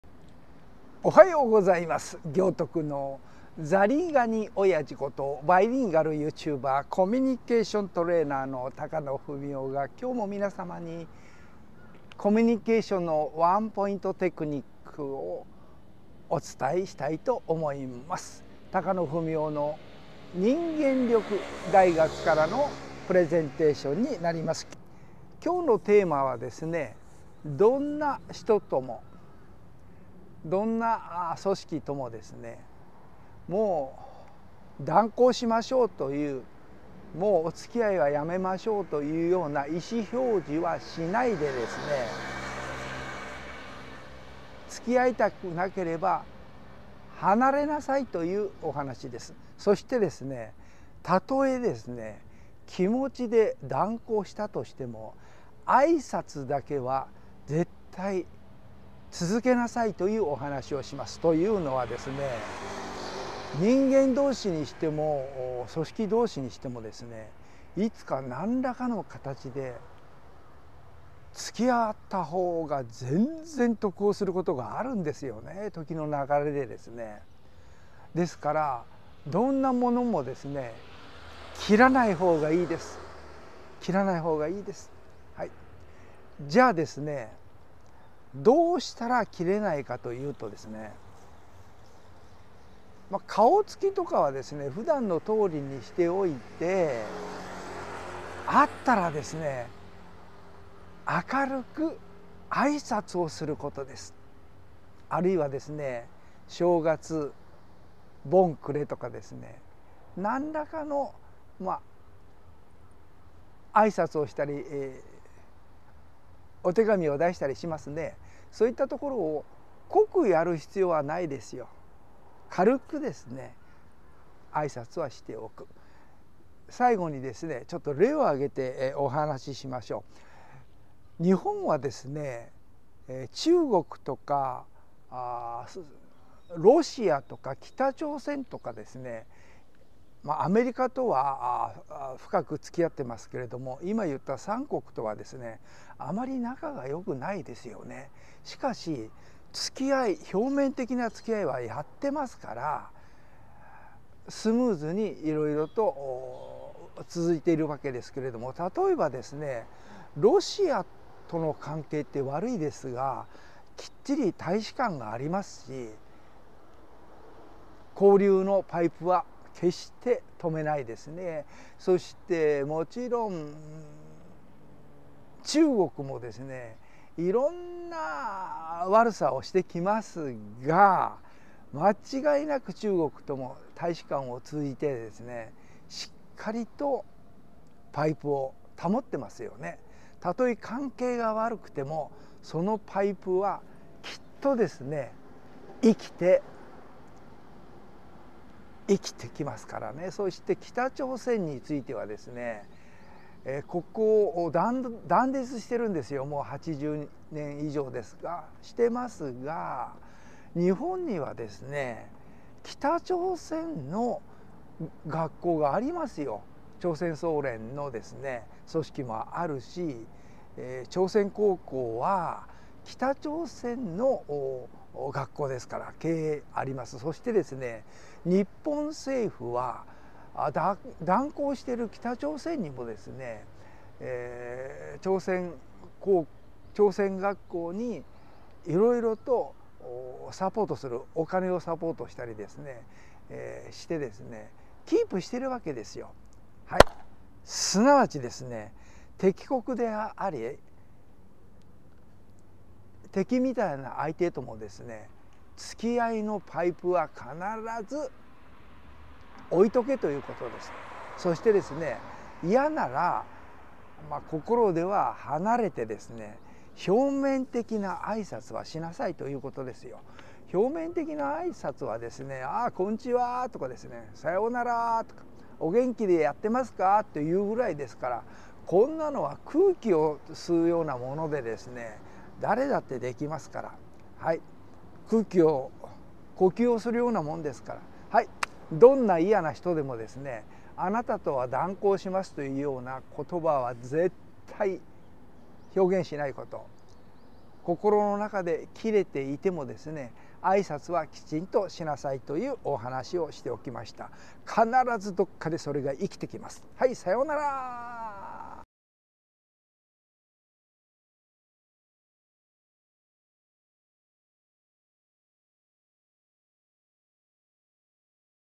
(4830) どんなに嫌いな人でも繋ぎの挨拶は忘れないで置く！
嫌いな人に、私はあなたと絶交したい！
と意思表示をしてもしょうがないと思う。

心で絶交しても、表面的には挨拶を怠ら
ず付き会って行けばよいのです。

世の中は広い様で狭いものです。
率先して敵を作る人要なんてどこにある
でしょうか？

挨拶を呼吸をしているように簡単に
出来るように自己訓練を積むことです。

確かに、それが簡単にはできなのが
人間なんですよね。

だから、人間学を学び、上手に生きる
呼吸のような物を学んで身に付けてる
のが教育の大切さだと思うんです。

私が長年訓練して来ました、武道や
武術なんかも、そのような生き方が
出来る様になる為の訓練だと思うんです。

教育 人生を学ぶ意義 学校教育 学び コミュニケーション プレゼン背Wondershare Filmoraで作成